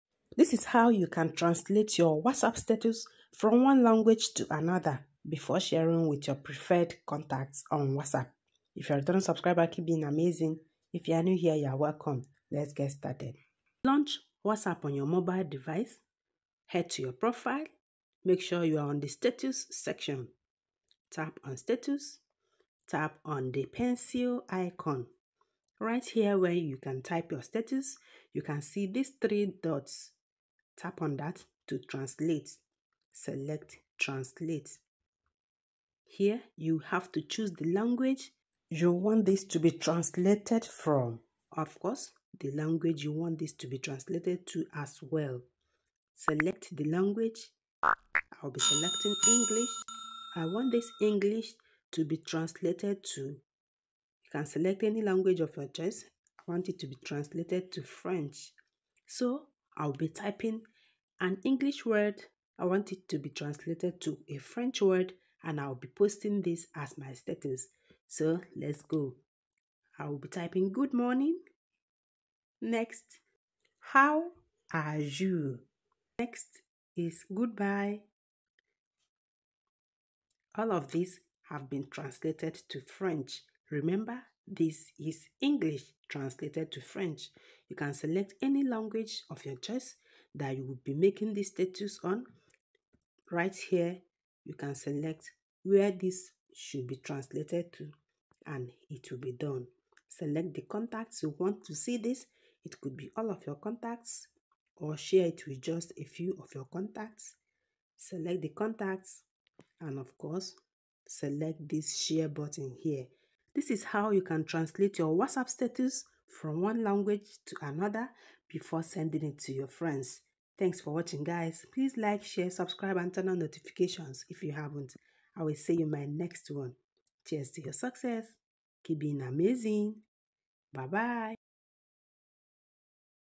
HOW TO TRANSLATE WHATSAPP STATUS
How to translate WhatsApp status from source language to target language
translate WhatsApp status from one language to another